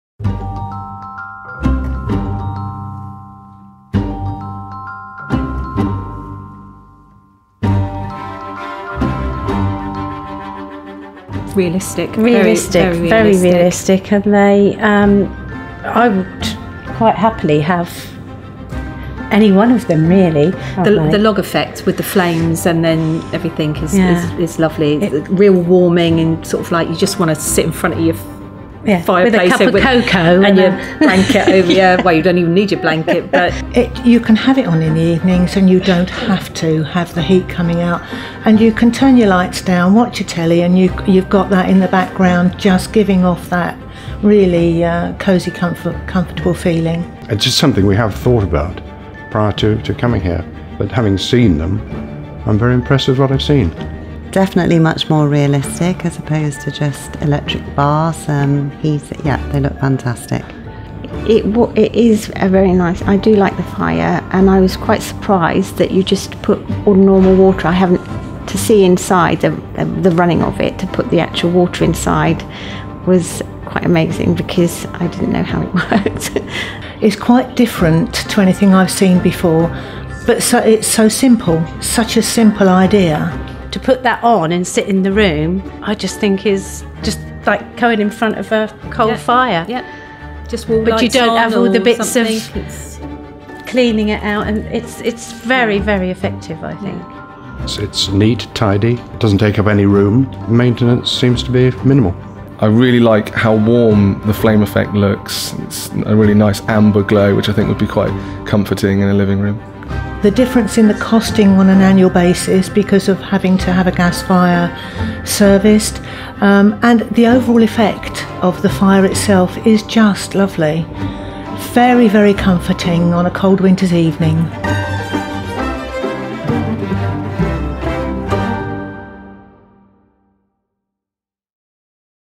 Opti-myst: What consumers think
Dimplex Opti-myst captivates everyone who sees it for the first time.  See what these consumers think of Opti-myst, featuring the world’s most realistic 3D electric flame effect .

Dimplex is the market leader in electric heating & renewables; hundreds of trusted products for energy efficient heating.